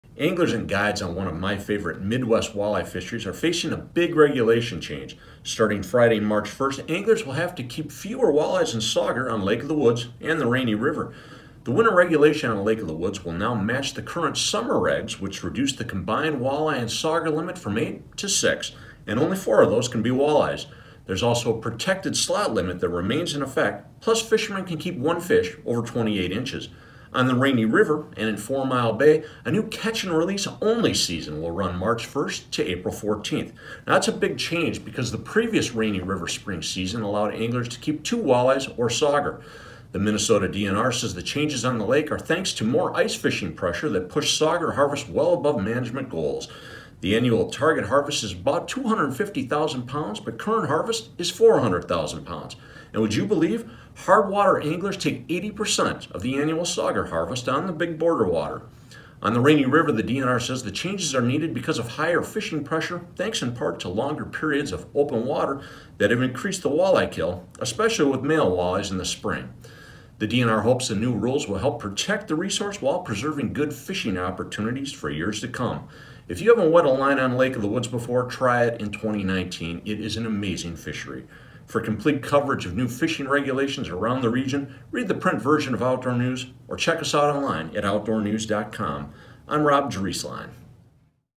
2019 Lake of the Woods Regulations (Walleye)
Rob Drieslein, Managing Editor at Outdoor News, shares the latest regulation changes on the Rainy River and Lake of the Woods.